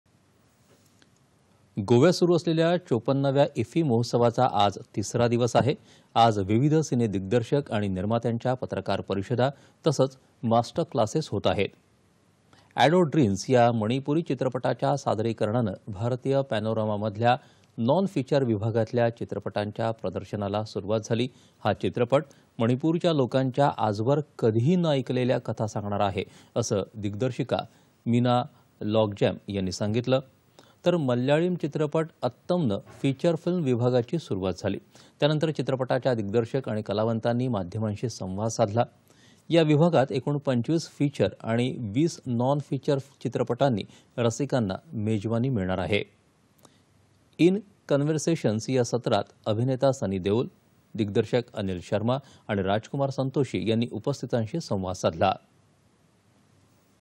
गोव्यात सुरु असलेल्या ५४ व्या इफ्फी महोत्सवाचा आज तिसरा दिवस आहे.
दि, २२.११.२०२३ :
       गोव्यात सुरु असलेल्या  ५४ व्या इफ्फी महोत्सवाचा आज तिसरा दिवस आहे. आज विविध सिनेदिग्दर्शक आणि निर्मात्यांच्या पत्रकार परिषदा तसंच मास्टर क्लासेस होत आहेत.
ॲडो ड्रिन्स या मणिपूरी चित्रपटाच्या सादरी करणानं भारतीय पॅनोरामामधल्या नॉन फिचर विभागातल्या चित्रपटांच्या प्रदर्शनाला सुरुवात झाली.हा चित्रपट मणिपूरच्या लोकांच्या आजवर कधीही न ऐकलेल्या कथा सांगणारा आहे, असं दिग्दर्शिका मीना लॉगजॅम यांनी सांगितलं. तर मल्याळ चित्रपट अत्तम नं फिचर फिल्म विभागाची सुरुवात झाली. त्यानंतर चित्रपटाच्या दिग्दर्शक आणि कलावतांनी माध्यमांशी संवाद साधला.  या विभागात एकूण 25 फीचर आणि 20 नॉन-फीचर चित्रपटांची रसिकांना मेजवानी मिळणार आहे.
    इन क्नव्हर्सेशन या सत्रात अभिनेता सनी देओल, दिग्दर्शक  अनिल शर्मा आणि राजकुमार संतोषी यांनी उपस्थितांशी संवाद साधला.

8.30am | 11.00am | 1.00pm | 4.30pm | 7.00pm | 9.30pm